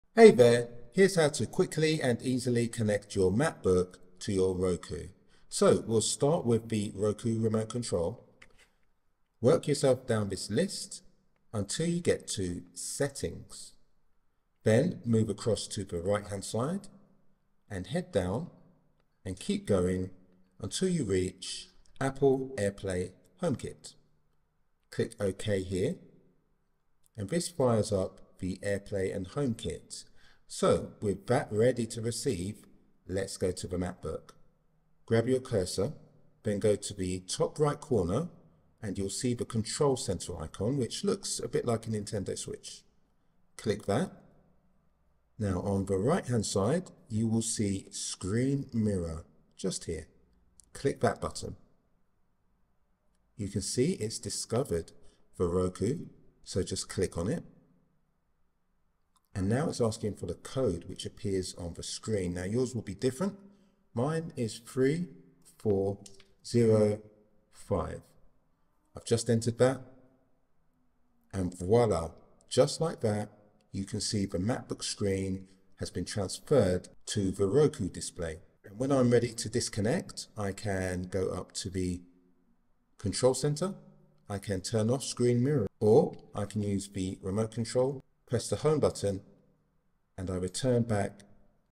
How To Connect MacBook to Roku
Here's how to connect your MacBook to your Roku. Step by step instructions. Both devices need to be connected to the same WIFI.

Share your MacBook on your Roku in next to no time.

iPad Pro
iPad Air
Apple Watch Ultra 2
Samsung Tab A9
Anker USB Hub

Why not share this with a friend?